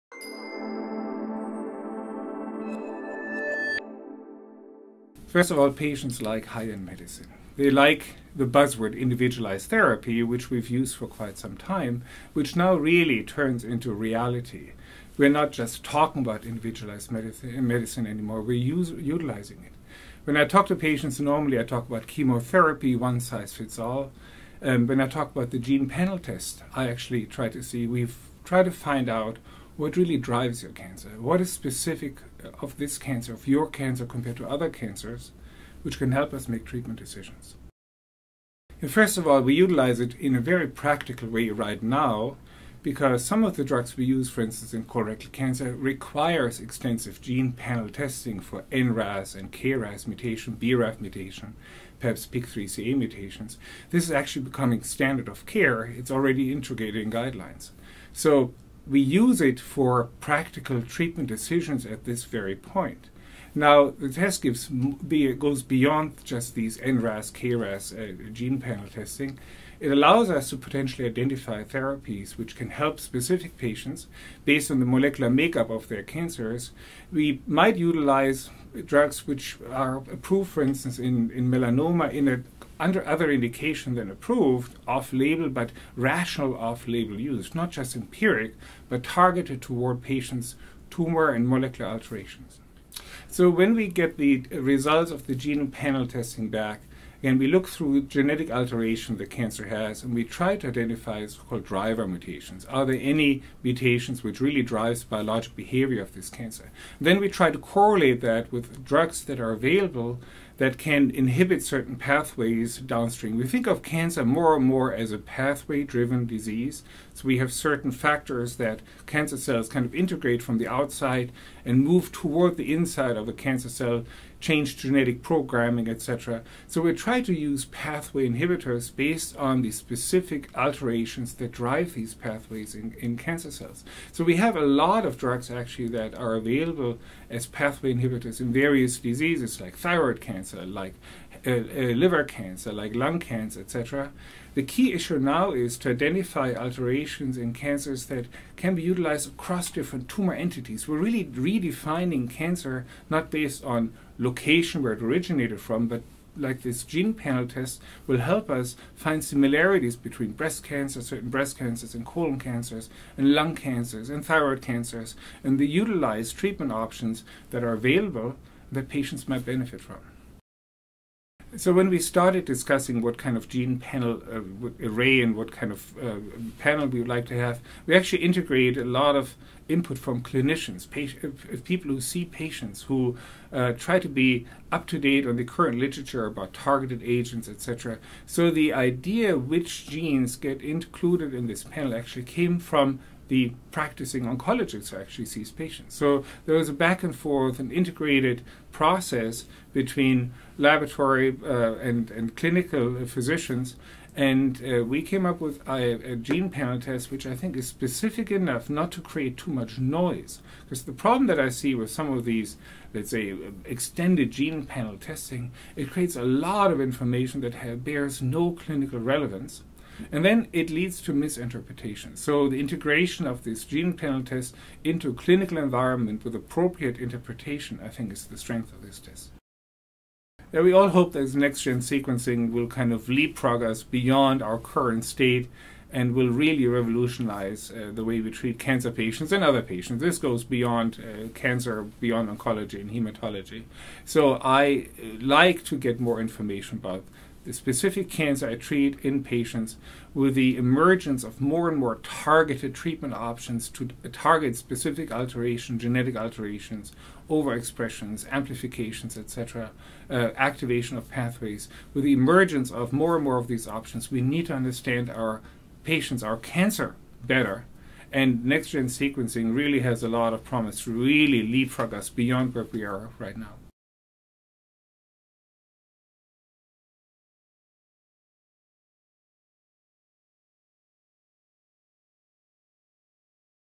Next Generation Sequencing Panel for Solid Tumor Cancers - A Test in Focus
Axel Grothey, M.D., discusses a new gene panel cancer test to help tailor chemotherapy to the individual patient based on the unique genomic signature of the patient's tumor. This test scans specific regions in 50 genes known to affect tumor growth and response to chemotherapy. The test is now available to Mayo Clinic patients and to providers worldwide through Mayo Medical Laboratories.